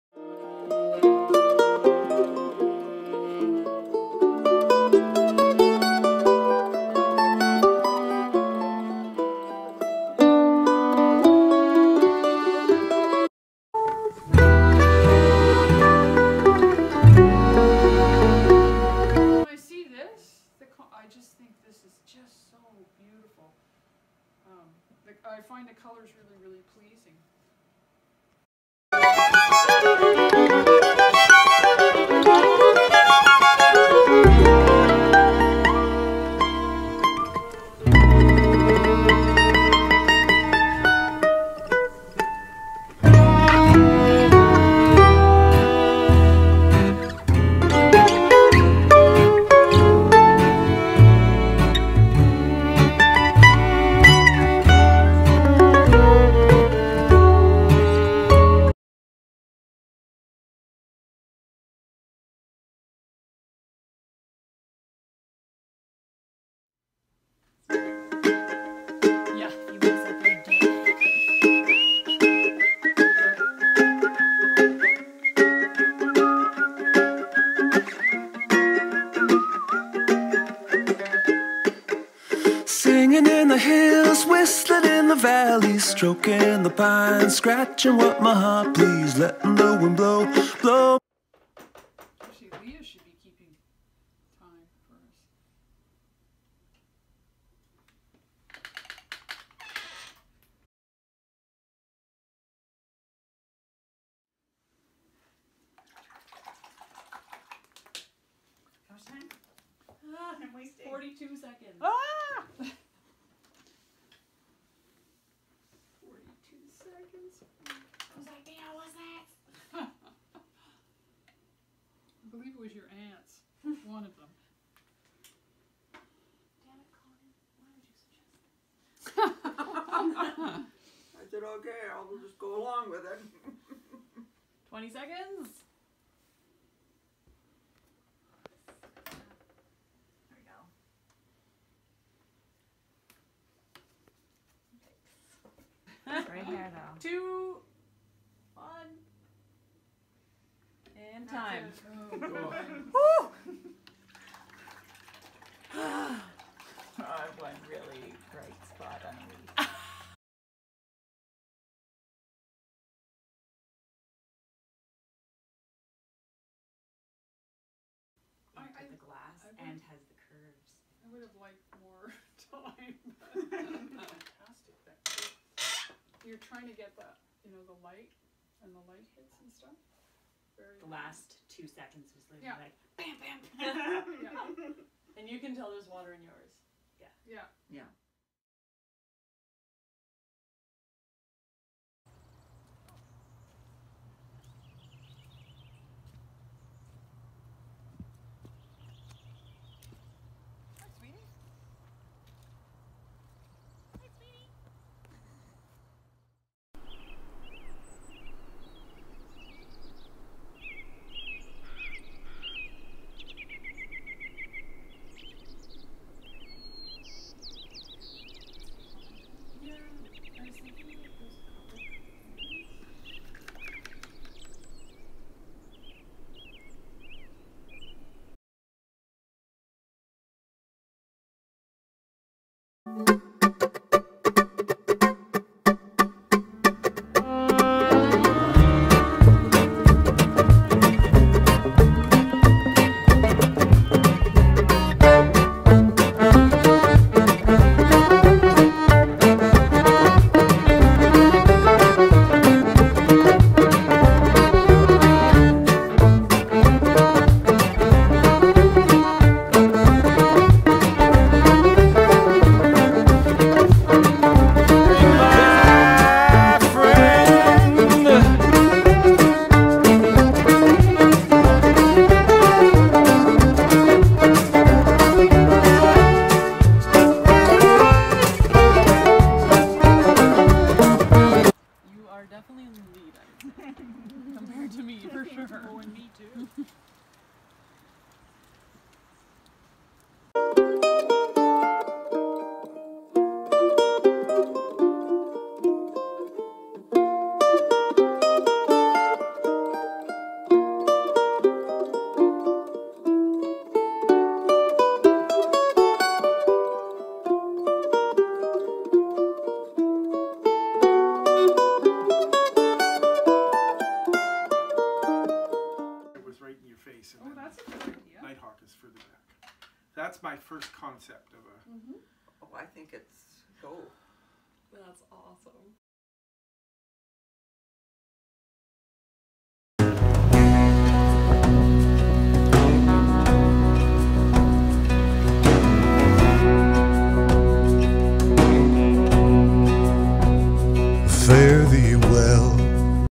Leaning Oaks Art Residency 2022
The family artists are back! If you want to hear their discussions, sound has to be up. Way up.
Music clips from The Bills and Stephen Fearing.